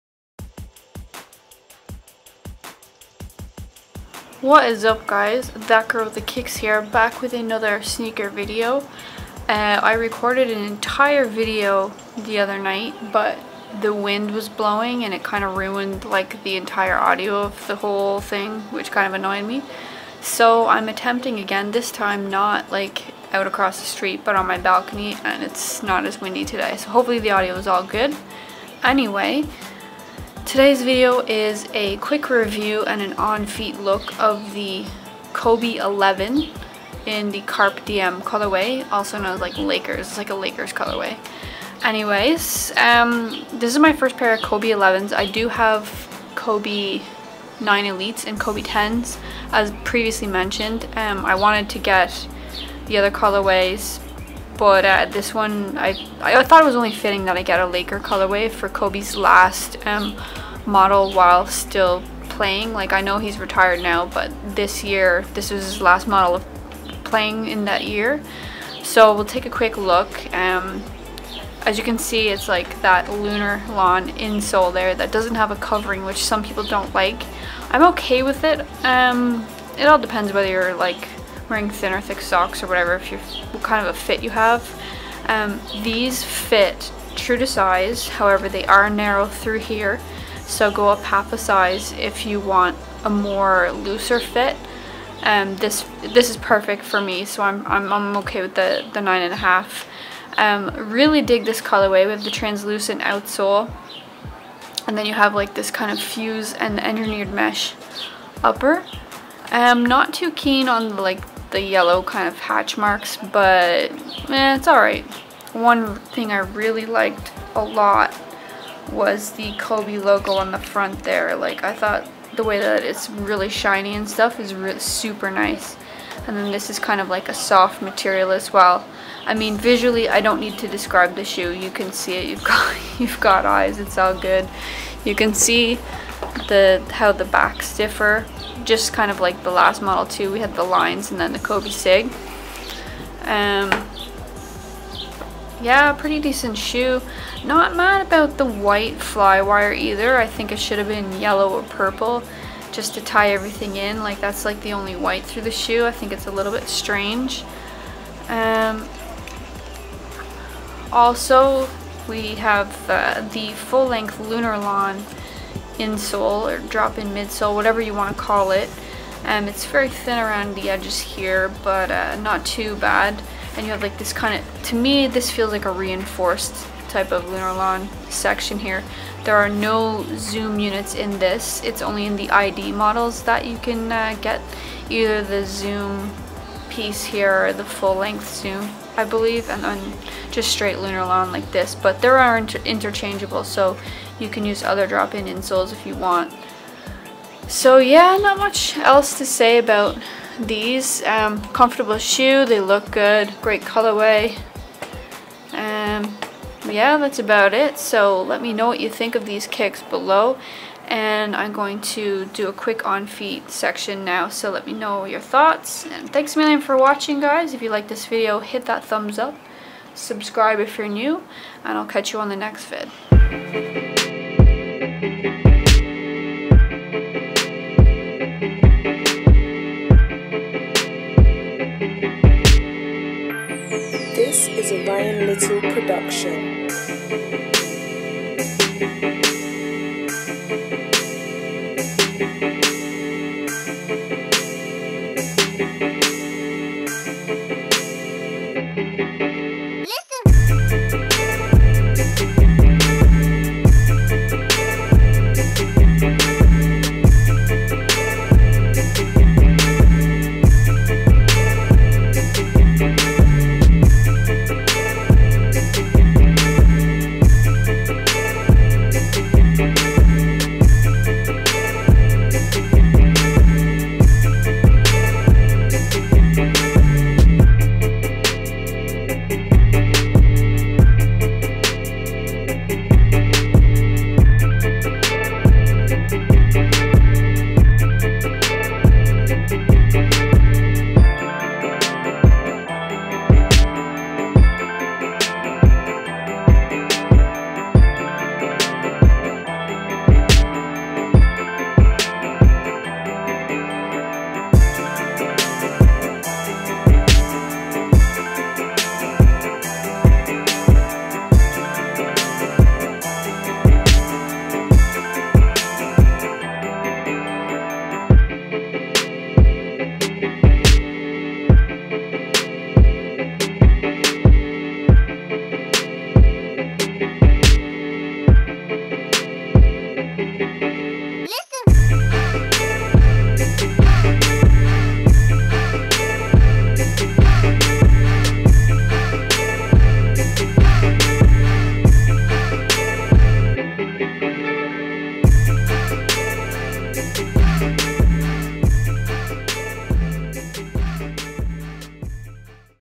Nike Kobe XI (11) Carpe Diem Review + On Feet Look
Quick thoughts on the kicks, plus an on feet look.